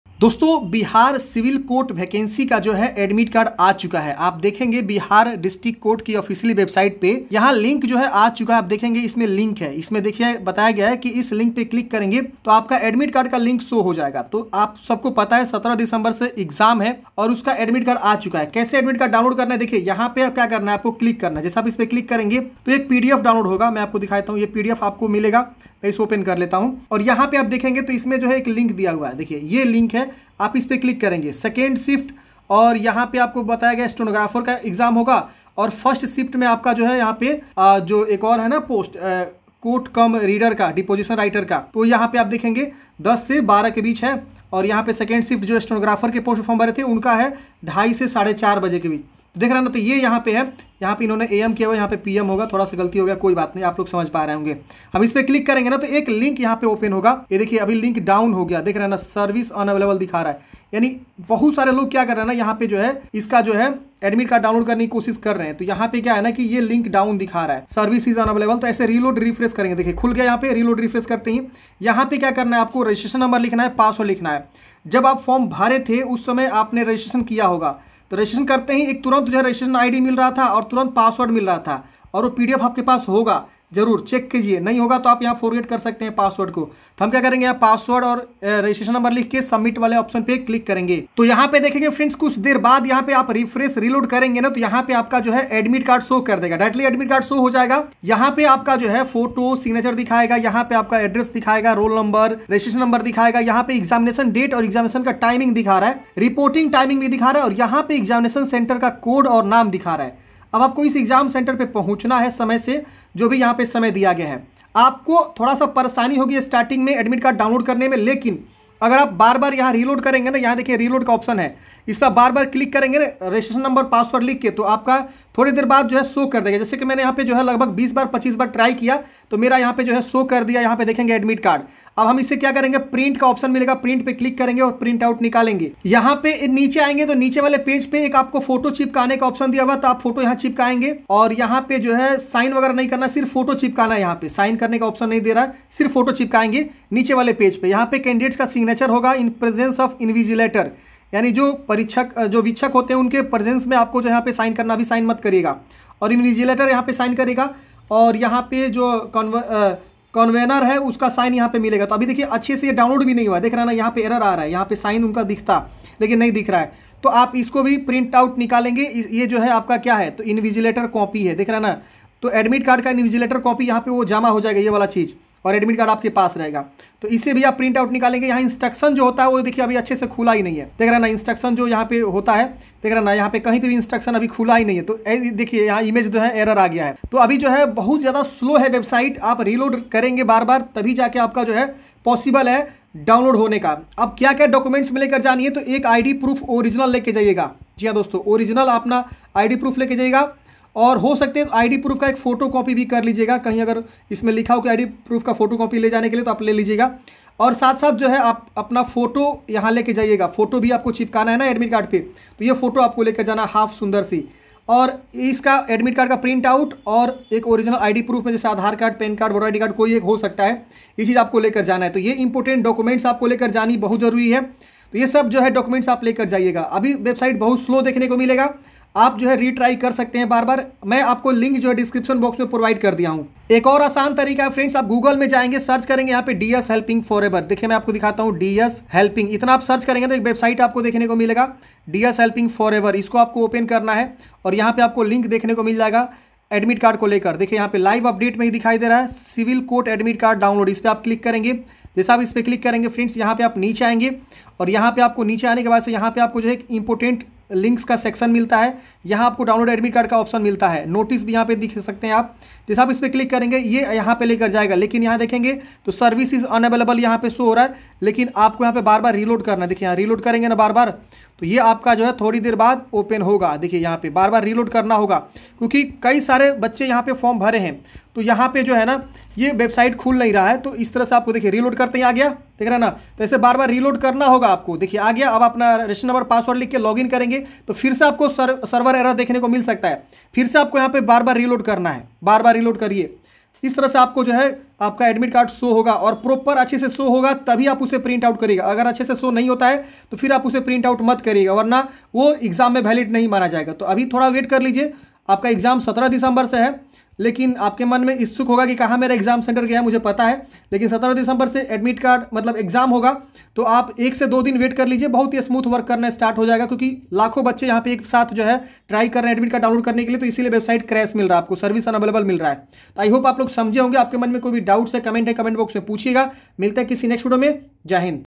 Bihar Civil Court Admit Card 2023 Download Kaise Kare | How to download Bihra Civil Court Admit Card
In this video I talka about Learn Bihar civil court admit card 2023 download kaise kare. How to download Bihra Civil Court Admit Card 2023 Step by Step with direct link. Now link is 100% active for Bihar Civil Court Exam 2023.

Patna Civil Court Exam Date 2023: Patna Civil Court has released the Official Notification for Patna Civil Court Examination 2023. That all Candidate who have applied for Patna Civil Court Recruitment 2022 can check own exam roution.